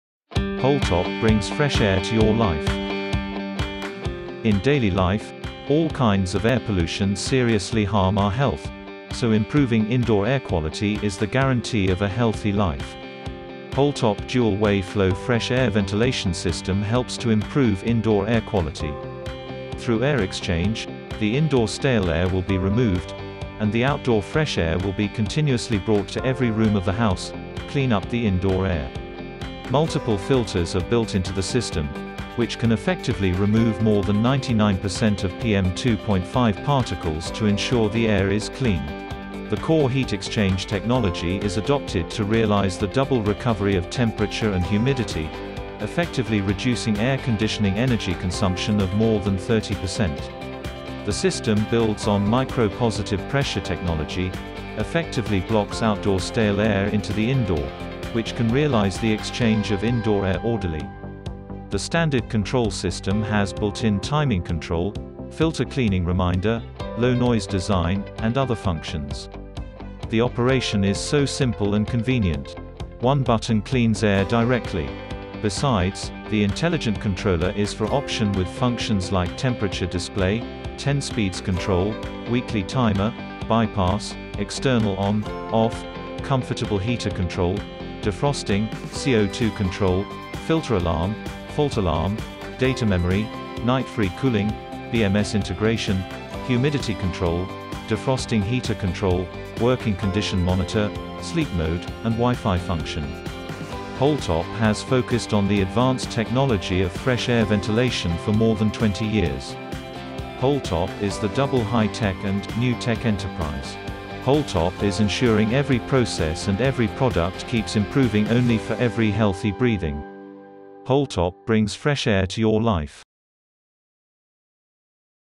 How Does an Holtop ERV Energy Recovery Ventilator Work?
Holtop dual-way flow fresh air energy recovery ventilator system helps to improve indoor air quality in an energy-saving way. Through air exchange, the indoor stale air will be removed, and the outdoor fresh air will be continuously supplied to every room of the house, clean up the indoor air.  Besides, Multiple filters are built into the system, which can effectively remove more than 99% of PM2.5 particles to ensure the air is clean. The core heat exchange technology is adopted to realize the double recovery of temperature and humidity, effectively reducing air conditioning HVAC system energy consumption by more than 30%.